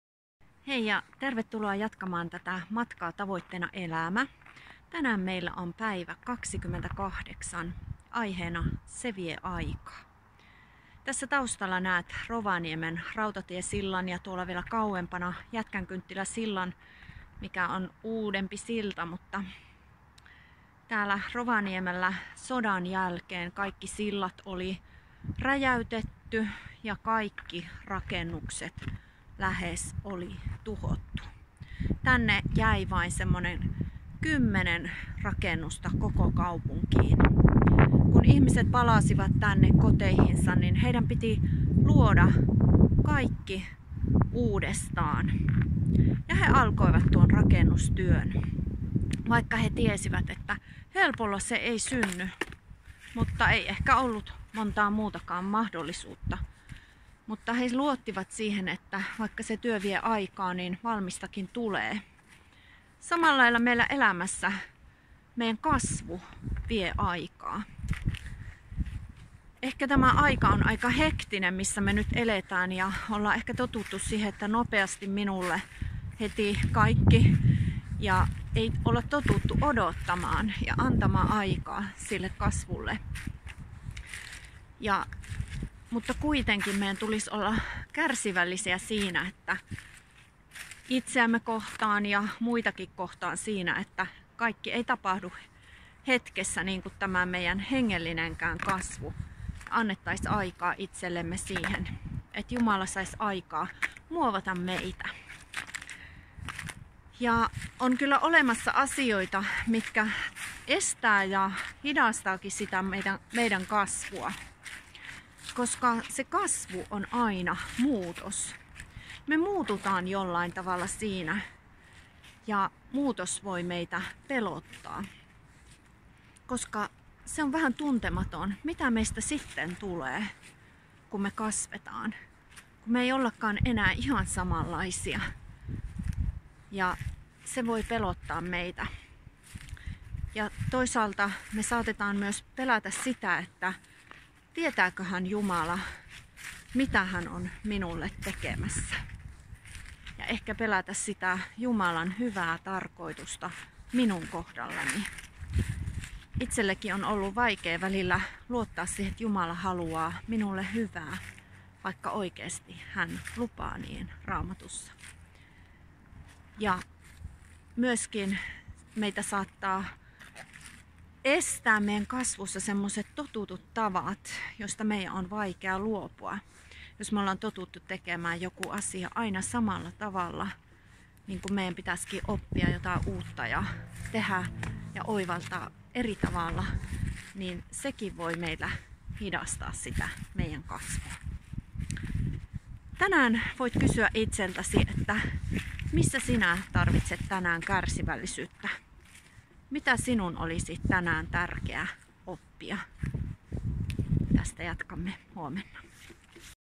Päivä 28 Se vie aikaa
Tule mukaan pohtimaan elämän tarkoitusta ja paikkaasi maailmassa. Pohdintojen peilinä Rick Warrenin kirja: Tavoitteena elämä. Teemme yhdessä 40 päivän matkan tähän teemaan, joka päivä tulee uusi video.